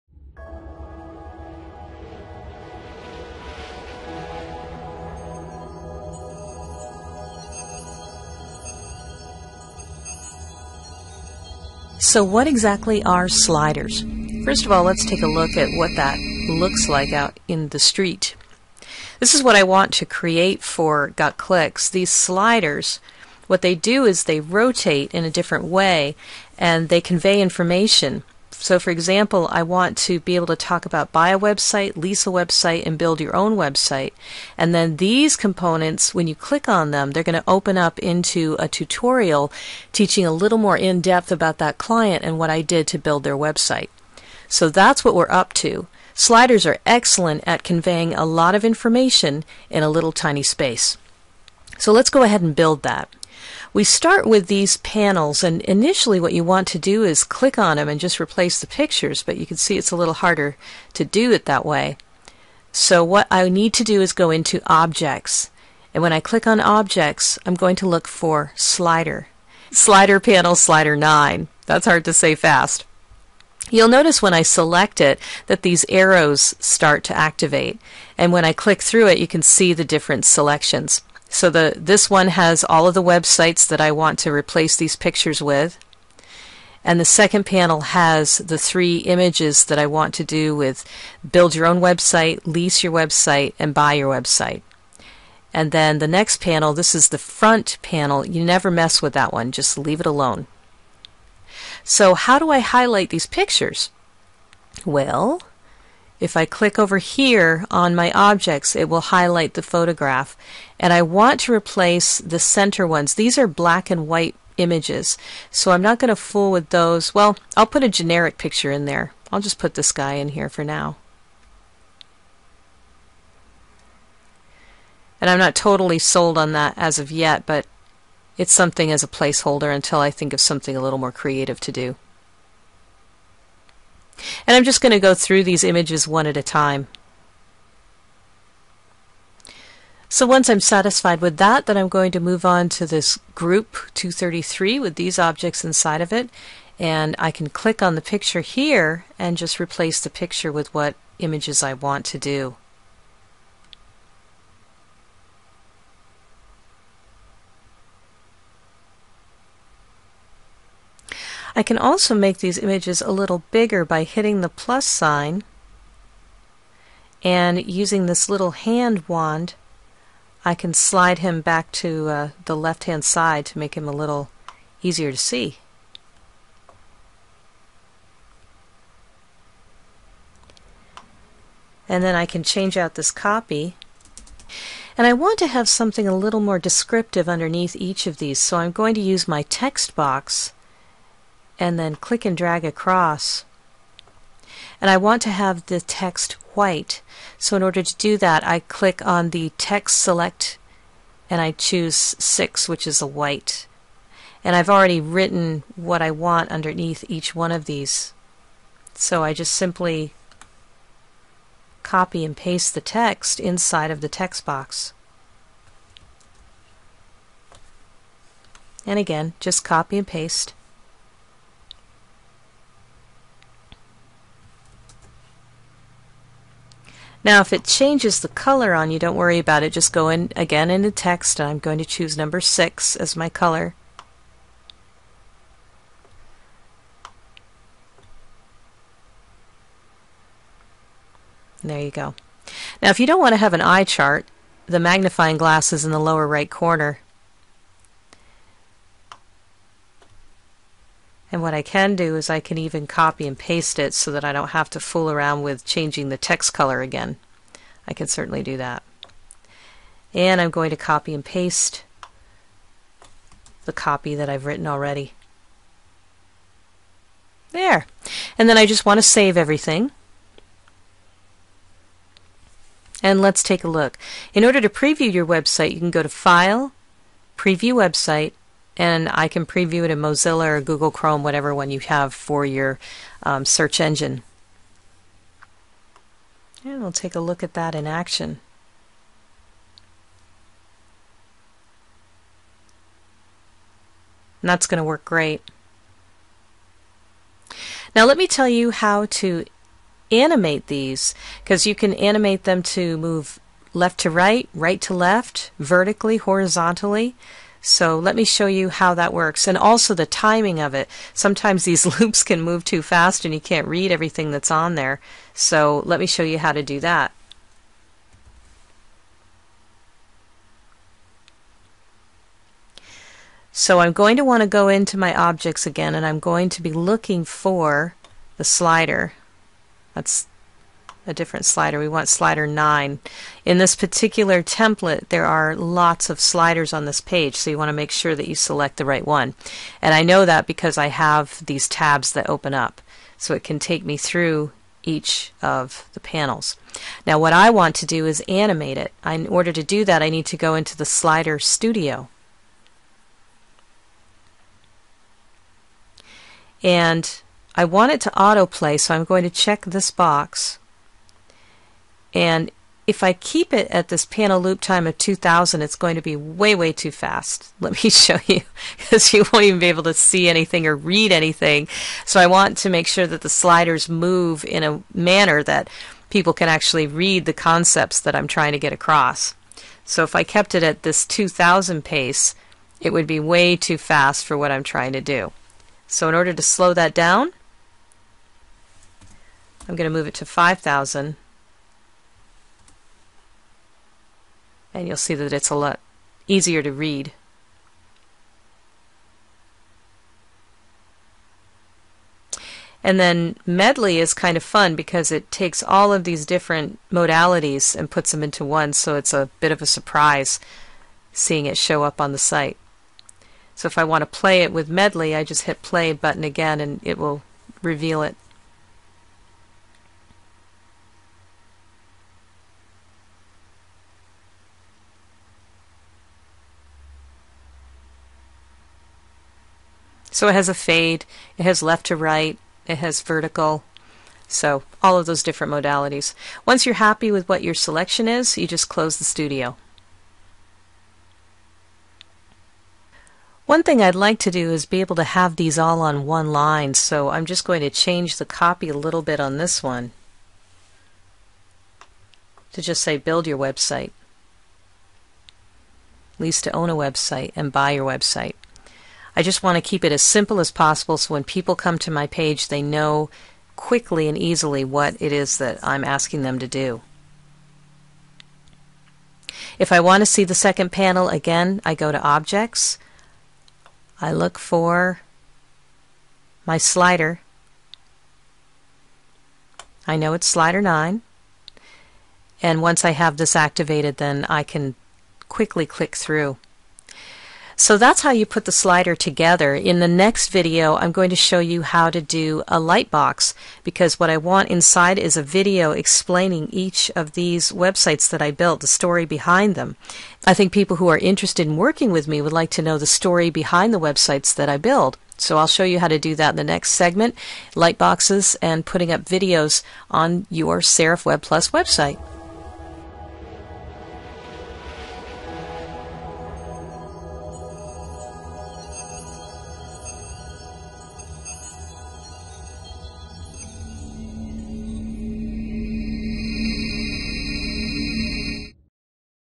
Serif WebPlus X8 Lesson 2 - Sliders and Panels
Sliders are a great way to convey a lot of information in a small space on your website.  Confusing at first, once you get the hang of how they work, they can really add extra zip to your Serif WebPlus X8 website.  If you'd like more free videos like this one, visit our